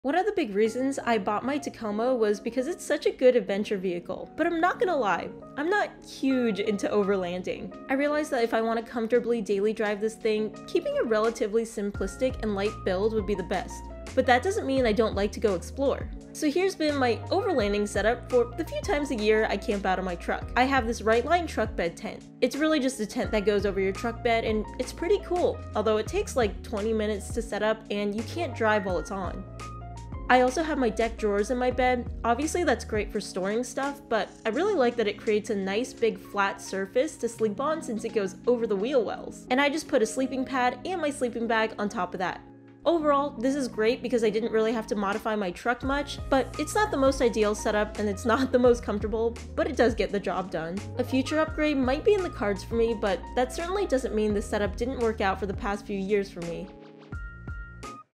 Camping with a Truck Bed Tent
In this I share my relatively simple overlanding/camping setup on my Tacoma!